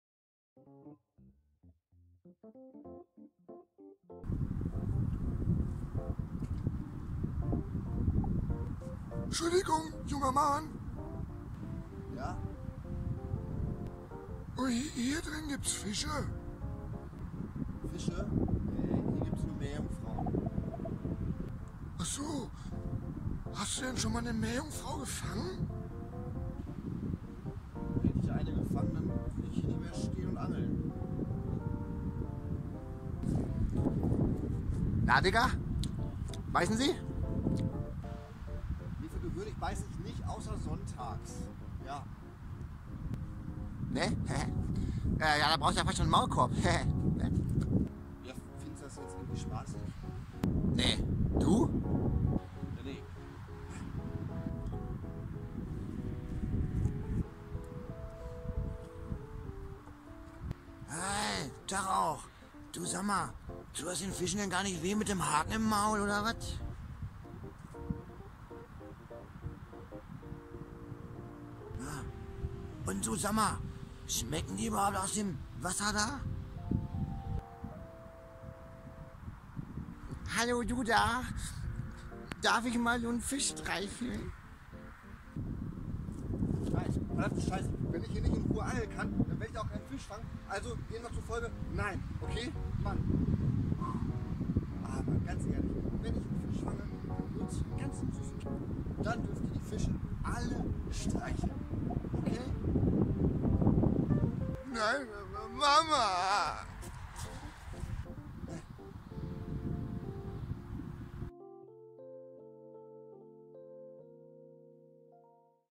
Des Anglers Alltag... Teil 2: " Spaziergänger"
Wer kennt sie nicht, die lieben "Nicht-Angler-Spaziergänger" ?!

An dieser Stelle viele Grüße an Alle :-)